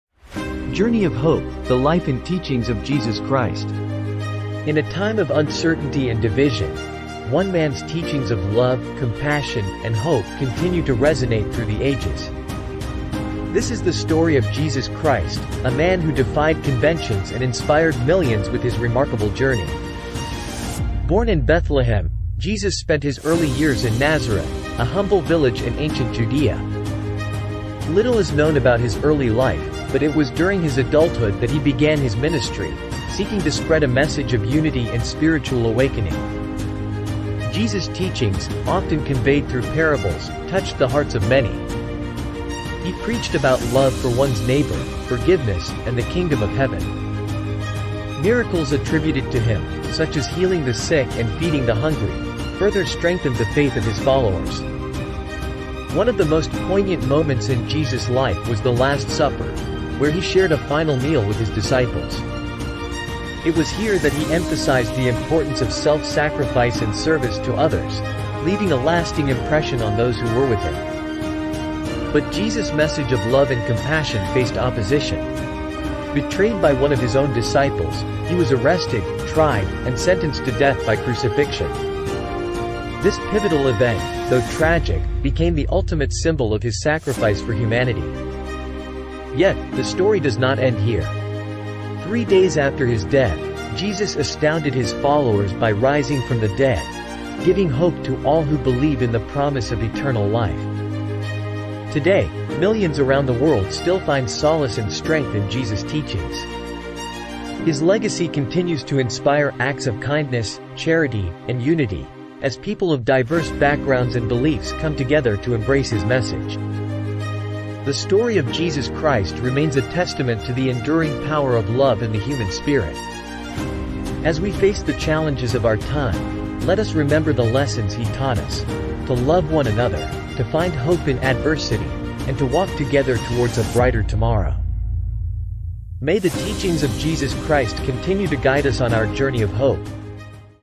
Journey of Hope: The Life and Teachings of Jesus Christ
In this video, we are sharing the Journey of Hope: The Life and Teachings of Jesus Christ. This video is a fascinating look at the life and teachings of Jesus Christ, and is sure to inspire you!

Whether you're a believer or not, this video is worth watching. It'll give you a greater understanding of the life and teachings of Jesus Christ, and may help you to make a difference in the world. I hope you enjoy this video!